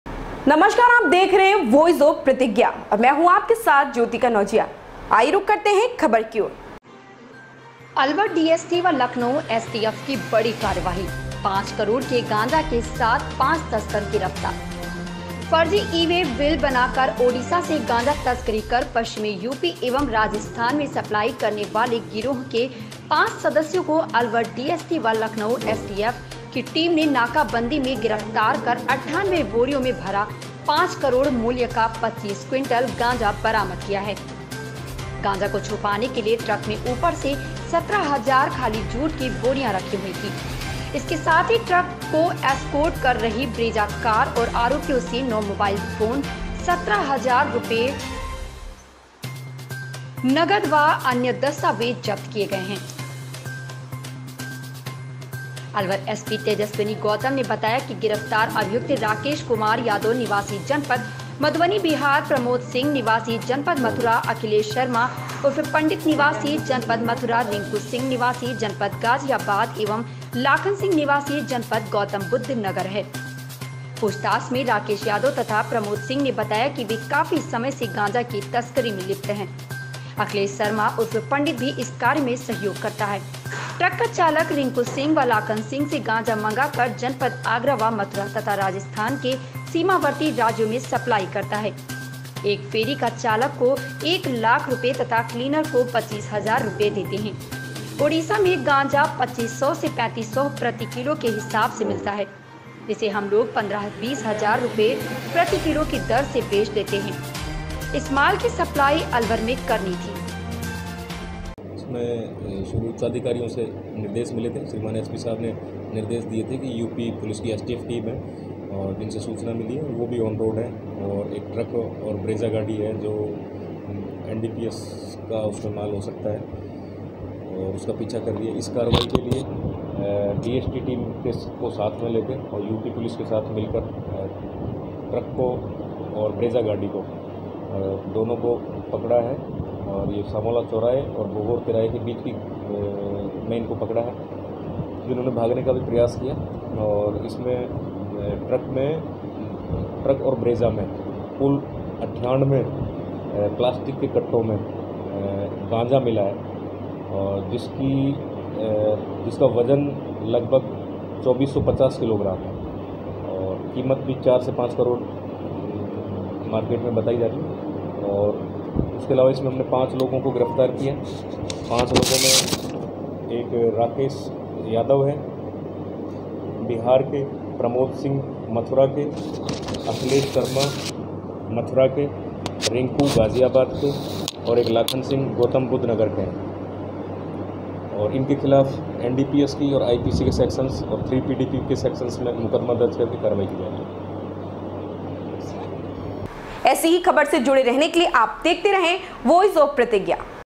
अलवर डीएसटी व लखनऊ एसटीएफ की बड़ी कार्रवाई, 5 करोड़ के गांजा से साथ 5 तस्कर गिरफ्तार
Video from pratigya alwar